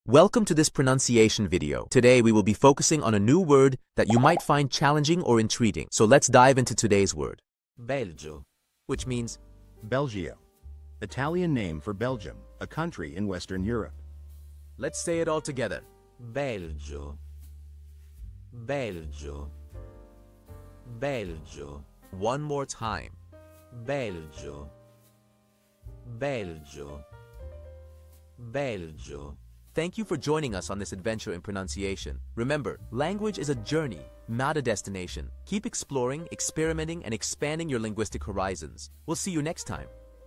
How to pronounce Belgio
Master the Pronunciation of 'Belgio Which Means Belgium' - which means : Belgium: Italian Name for Belgium, in Country in Western Europe. 📝 with @PizzaPronunciation 🎓🗣️ - [Your Guide to Italian]

Learn to pronounce 'Belgio' perfectly in Italian with our easy and engaging tutorial by @PizzaPronunciation! 📖📽️ In this comprehensive video, we provide you with native speaker examples 🗣️👂 and phonetic breakdowns 🎵 to ensure you get the pronunciation just right.

How to say Belgium in Italian